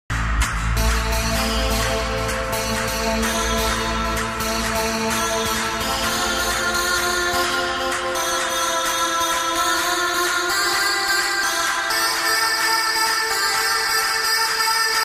New Body Fuel Paddy Pimblett Swizzels Drumstick Raspberry Flavour Applied Nutrition Drink
new from applied nutrition and endorsed by paddy the baddy pimblett a scouse UFC fighter

was out and about seen these amd grabbed it for £1.25